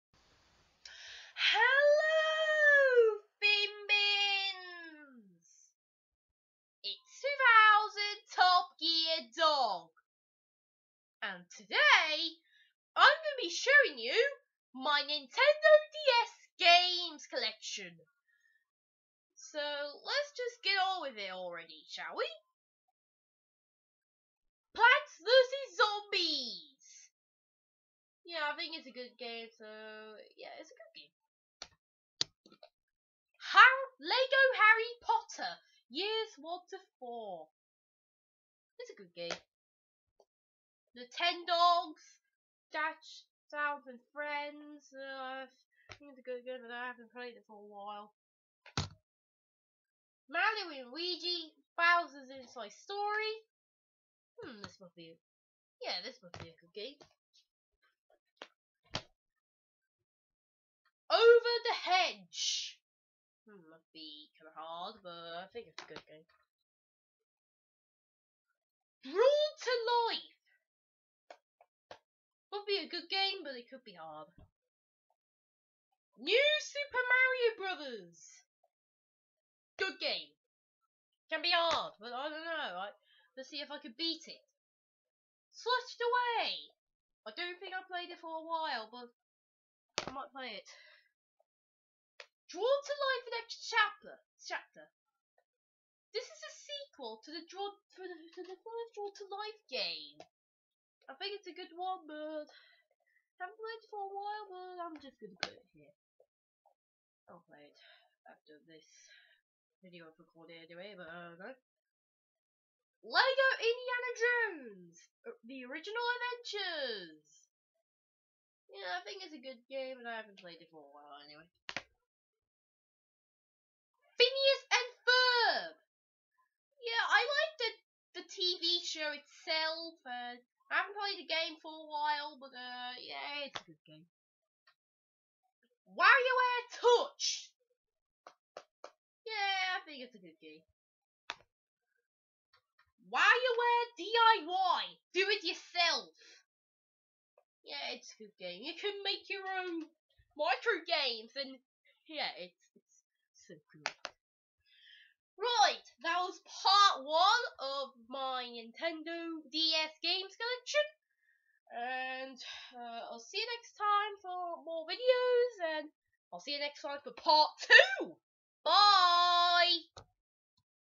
My Nintendo DS Game Collection Part 1
Hello, I am showing you my NDS game I have. This is Part 1.